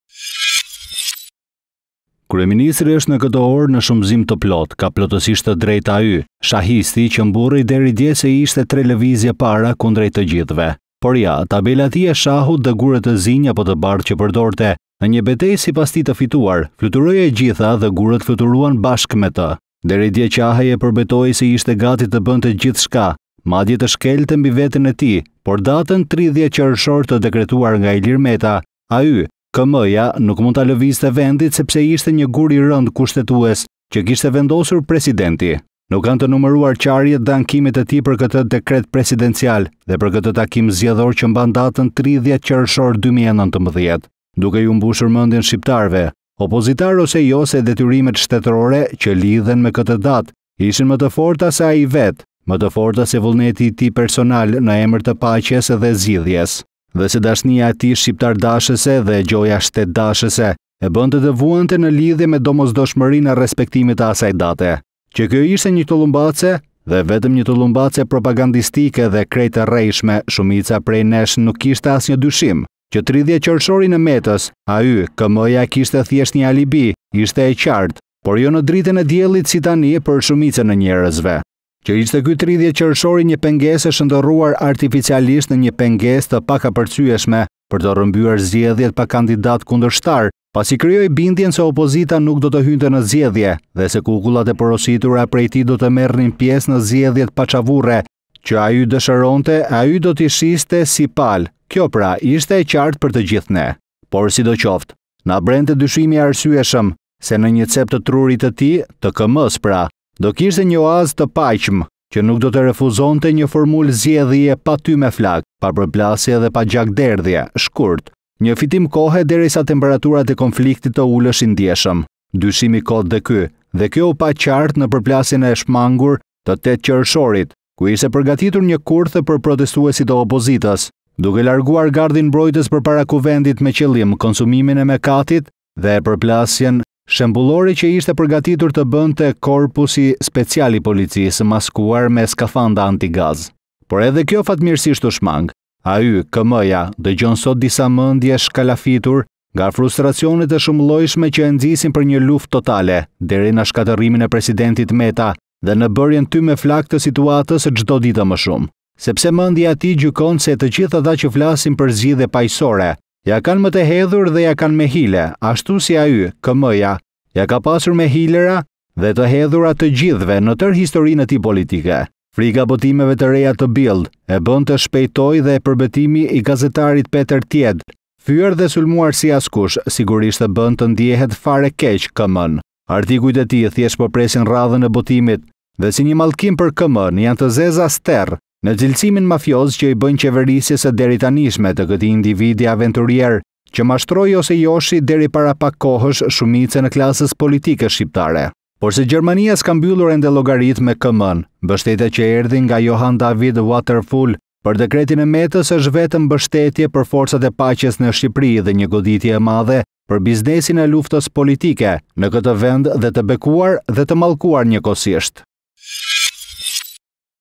Rama, grusht shteti?! Opinion nga Artur Zheji
Dhe se dashnija e tij shqiptardashëse dhe gjoja shtetdashëse, e bëntë të vuante në lidhje me domosdoshmërinë e respektimit të asaj date.
Që kjo ishte një tollumbace, dhe vetëm një tollumbace propagandistike dhe krejt e rrejshme, shumica prej nesh nuk kishte asnjë dyshim. Që 30 Qershorin e “Metës”, ay, Km-ja e kishte thjesht një alibi, ishte e qartë, por jo në dritën e diellit si tani, për shumicën e njerezve.